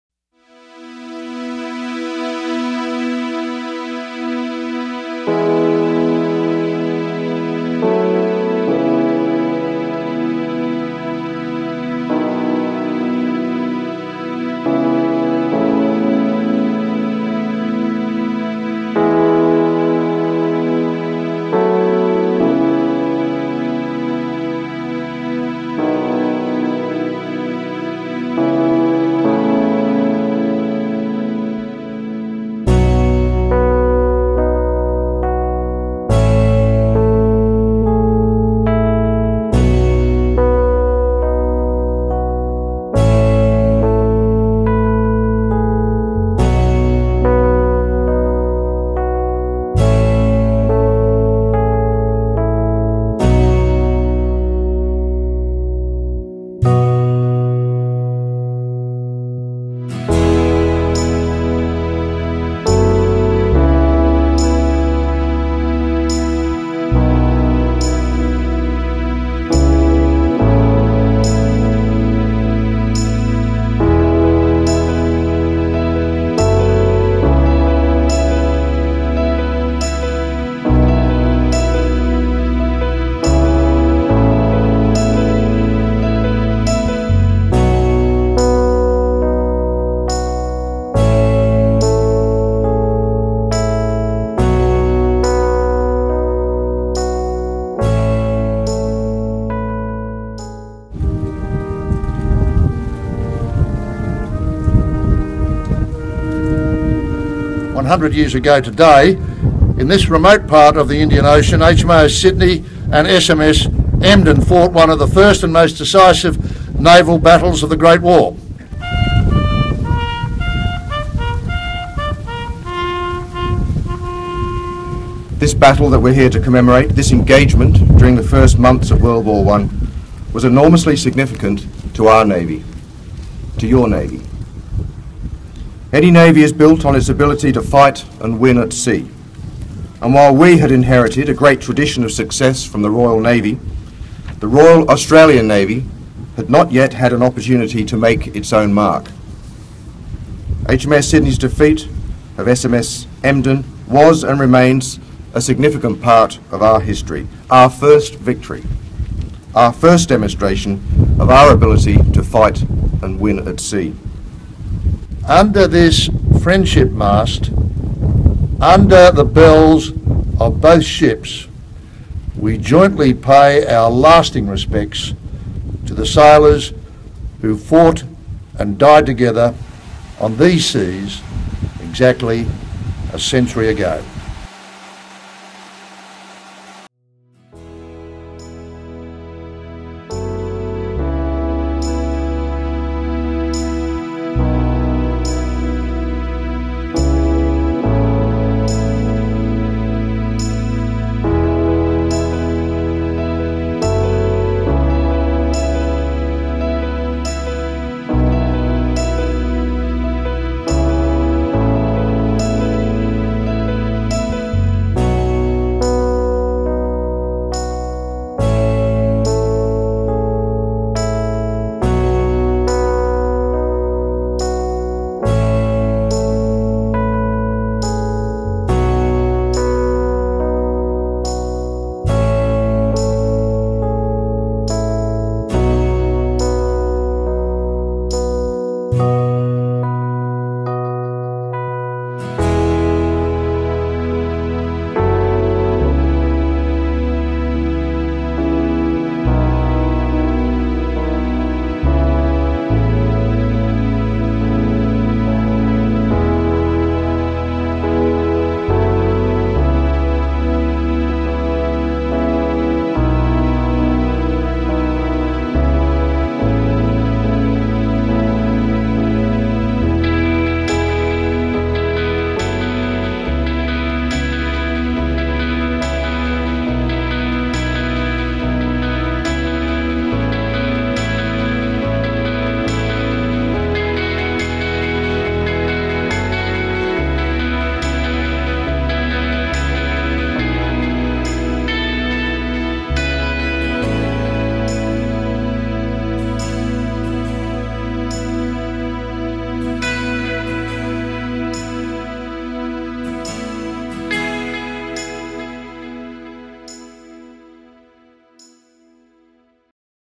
Sydney Emden 100 Commemorations
Between Friday 7th and Tuesday 11th November 2014, the Cocos Keeling Islands hosted the 'Weekend of Commemorations' for the 100 year anniversary of 'The Battle of Cocos'. On 9 November 1914 the HMAS Sydney I defeated the German raider SMS Emden, this was Australia's first naval battle victory.

Footage courtesy of The Royal Australian Navy
Images courtesy of attendees